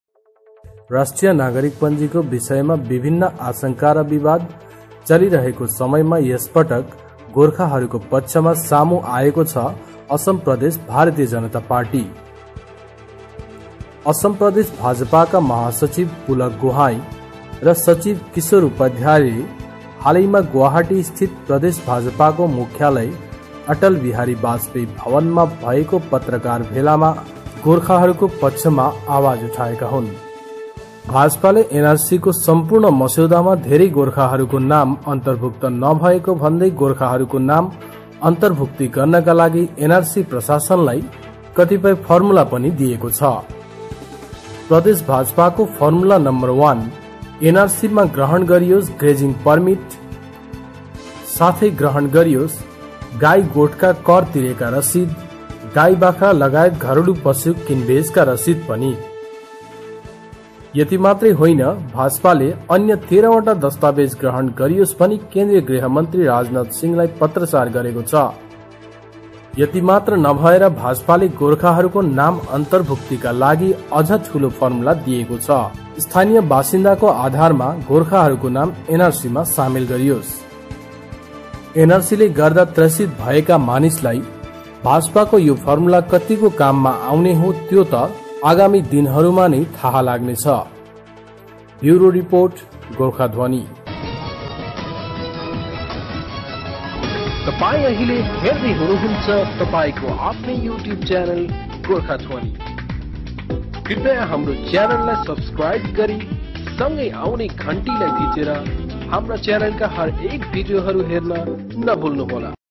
BJP Assam in Support of Gorkhas on NRC
The Assam State Bharatiya Janata Party(BJP) comes in front to support the Gorkhas residing in the state of Assam.
The State General Secretary Pulak Gohain and State Secretary Kishore Upadhyay addressed a Press Conference in the Atal Vihari Bajpayee Bhawan, Guwahati and supports the Gorkha people who are residing in the state in regards to the on going process of National Registers of Citizens(NRC).
BJP demanded to the NRC administration and the Co-ordinator Prateek Hajela to accept 13 additional documents including the Grazing Permit.